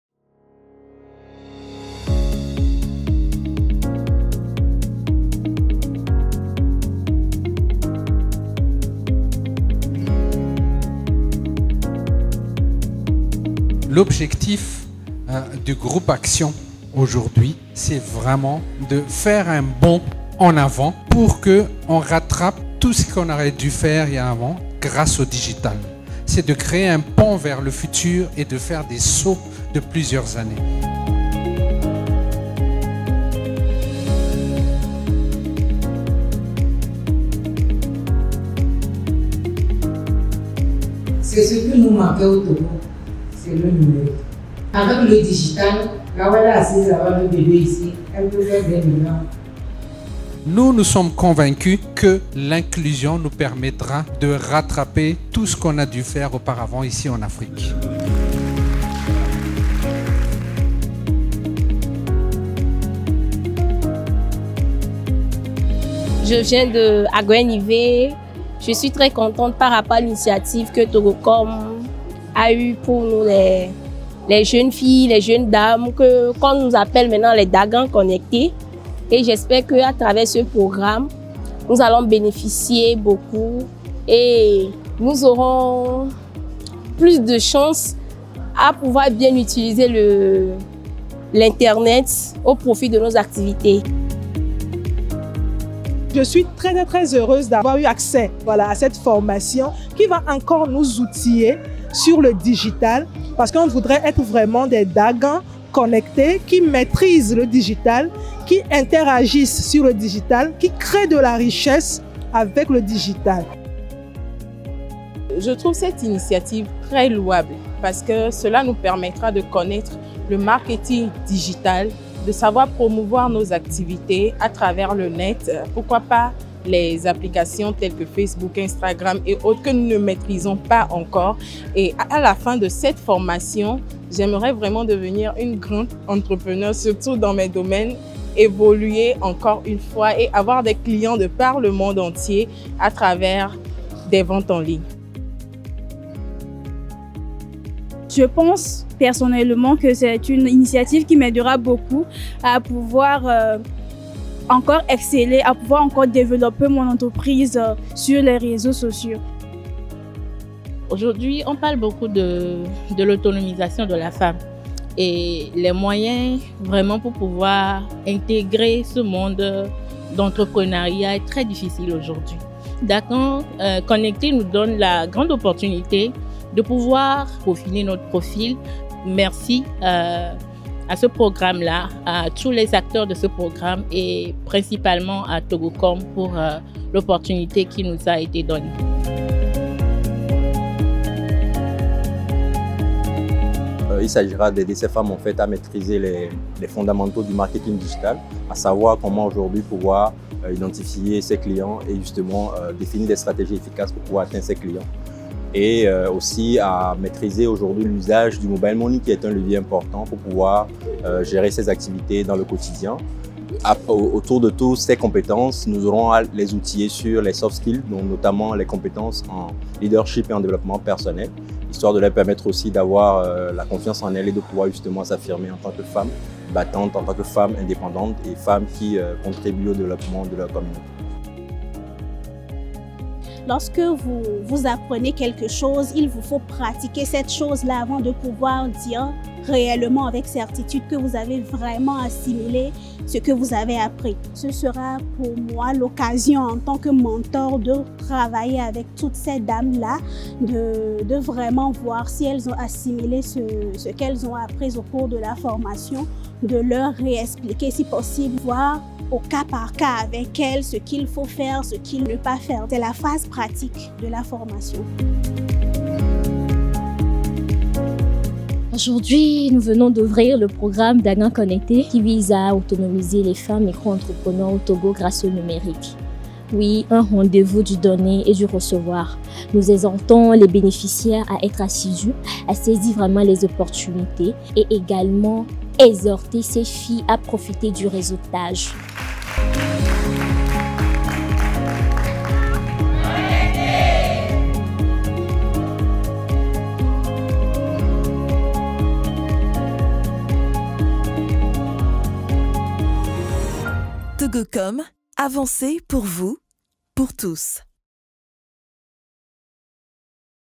Lancement de Dagan Connectée: 100 Femmes Micro-Entrepreneures au Togo avec Togocom !
Découvrez en vidéo le lancement des activités du programme Dagan Connectée, en présence de 100 femmes micro-entrepreneures bénéficiaires. 🌟

Cette cérémonie marquante a été l'occasion de rappeler l'objectif principal de Dagan Connectée : l'autonomisation économique des femmes micro-entrepreneures au Togo grâce au numérique. 💪🌐